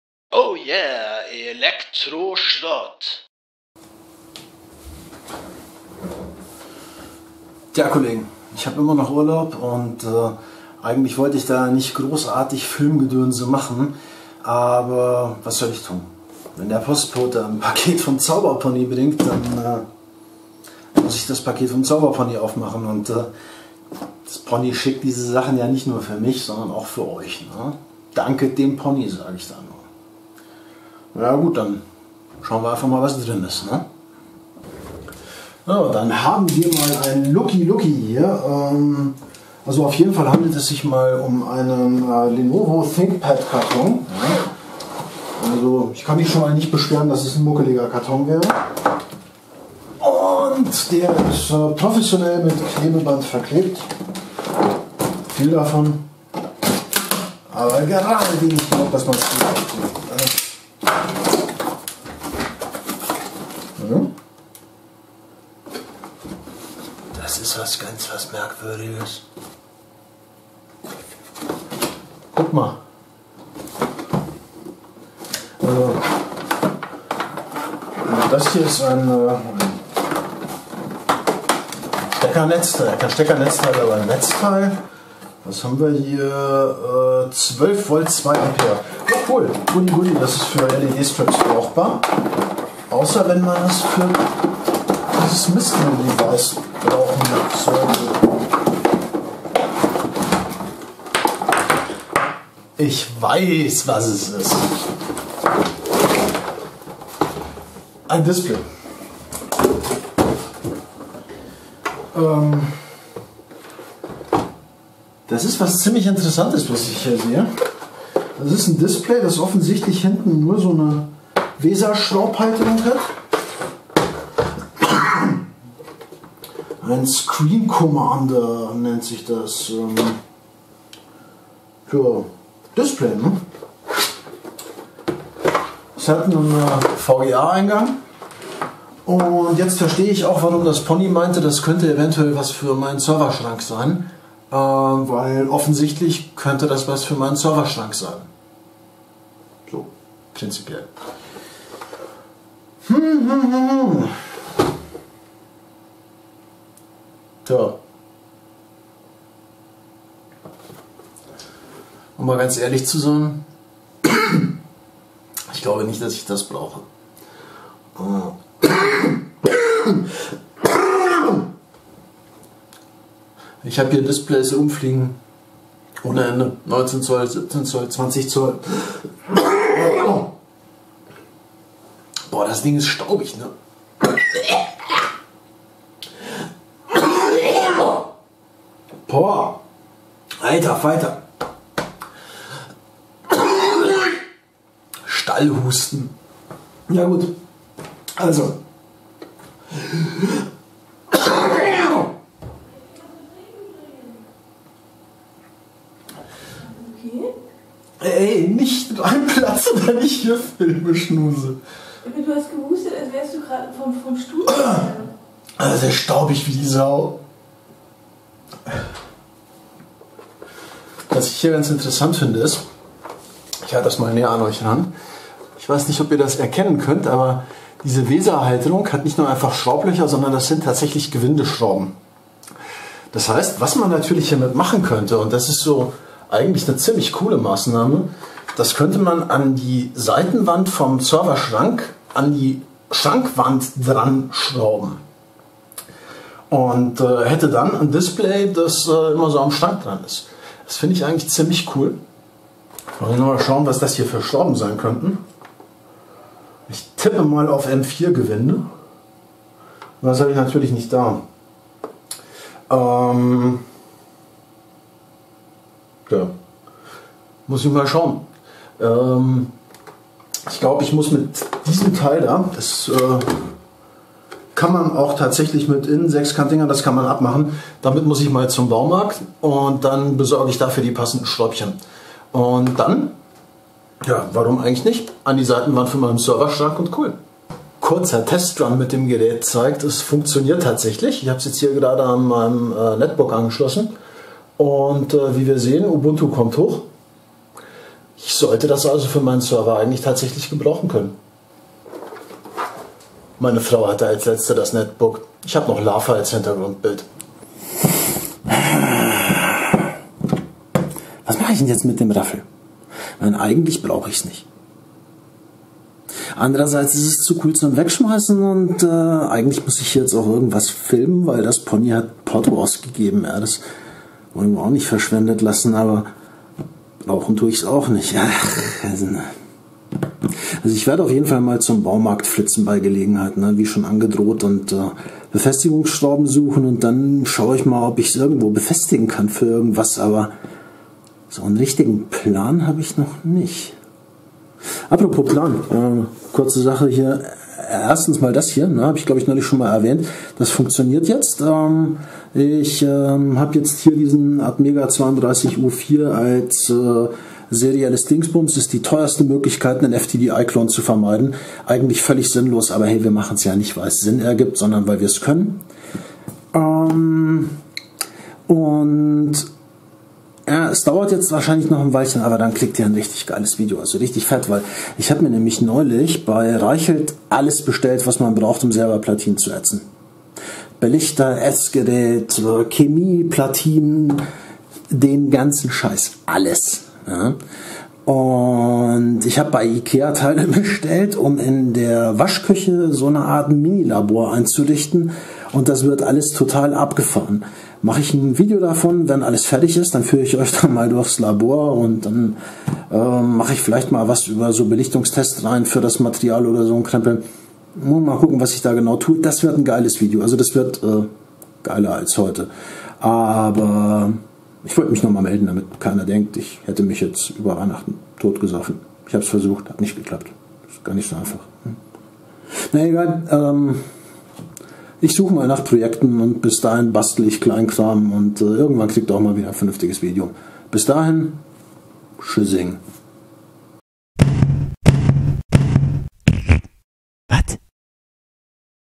Mehlbacke 7: Lungenstaub und Ponyscreen
Wenn der Postbote zweimal klingelt, habe ich das erste Klingeln mal wieder nicht gehört. Zum Glück habe ich es dann aber doch noch mitbekommen, und so könnt Ihr jetzt sehen, was mir das Zauberpony geschickt hat.

*** BETTEL-ALARM! ***

Dem Zappes sein Shop
Dem Zappes sein Wunschzettel